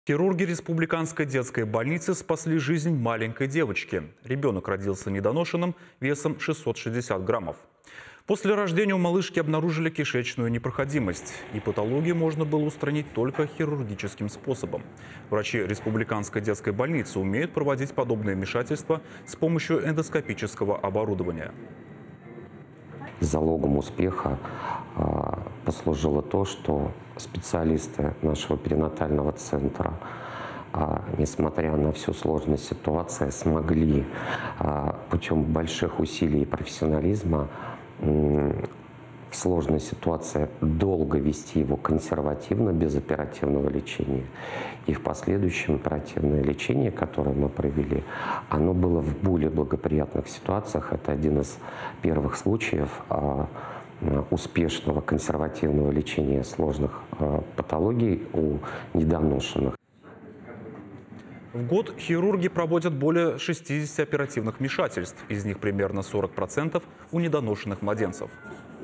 В Чувашии хирурги спасли жизнь недоношенной девочке весом 660 граммов
Хирурги Республиканской детской больницы спасли жизнь маленькой девочки. Ребенок родился недоношенным весом 660 граммов.